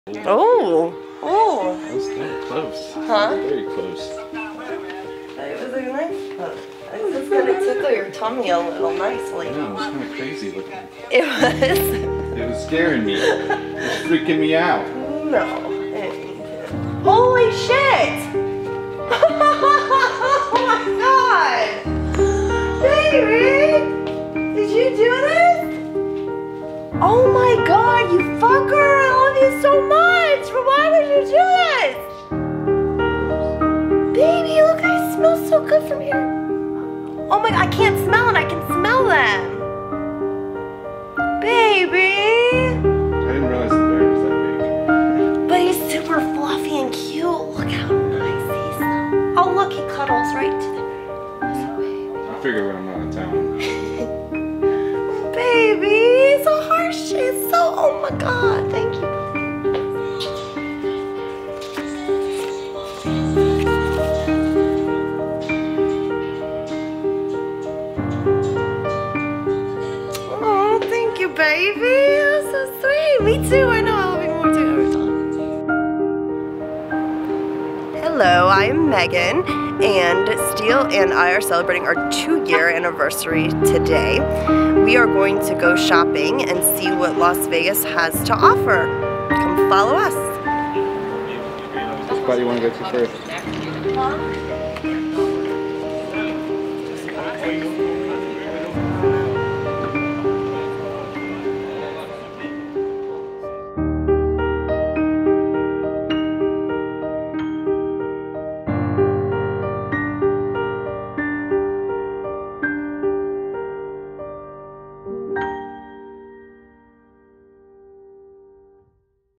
GH4 Short Film - Two Years
Today I celebrated two years with Megan and she hasn't tried to kill me.  Not once.  Thank you baby for all you put up with from me.